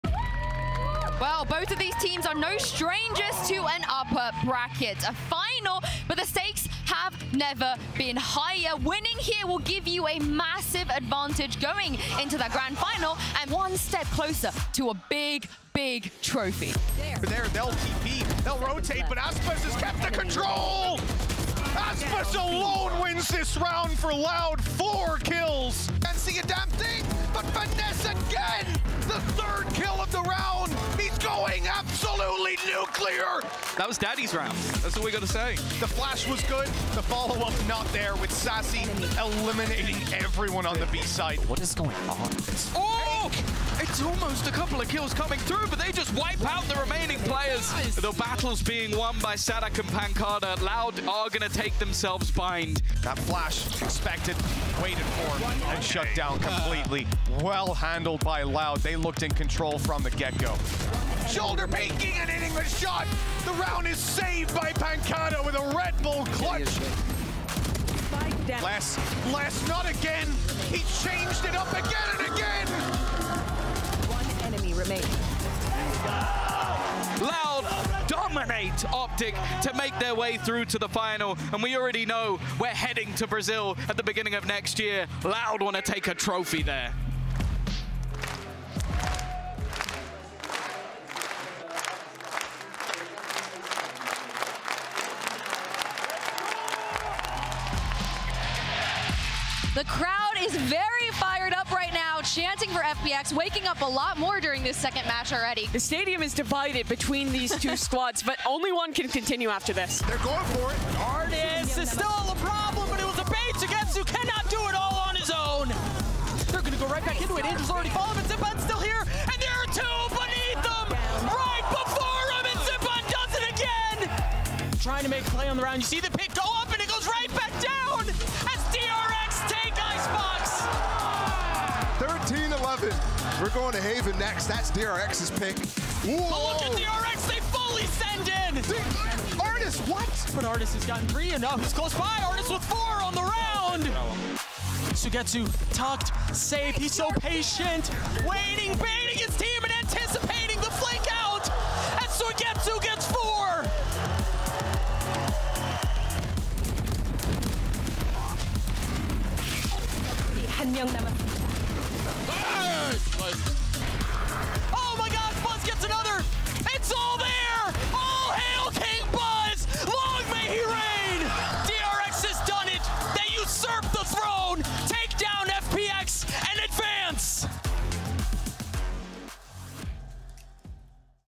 LOUD Punches Their Ticket To The Grand Finals | Champions Day 14 Recap
Day 14 is in the books. With a ticket to Grand Finals on the line, we see players pull out amazing plays to keep their dream alive. Relive the best moments.